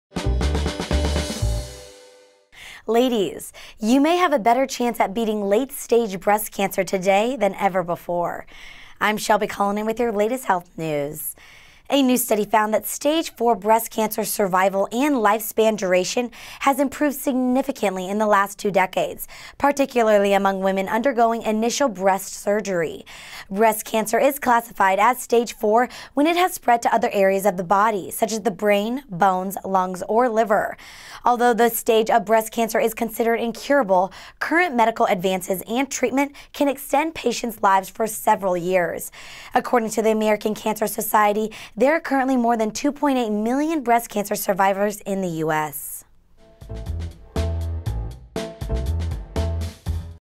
A Glimmer of Hope for Advanced Breast Cancer
Ladies, you may have a better chance at beating late-stage breast cancer today than ever before.

A new study found that stage IV breast cancer survival and lifespan duration has improved significantly in the last two decades — particularly among women undergoing initial breast surgery.

Breast cancer is the second most common cancer among women in the US. About 1 in 8 US women will develop invasive breast cancer at some point during her lifetime, according to the American Cancer Society (ACS).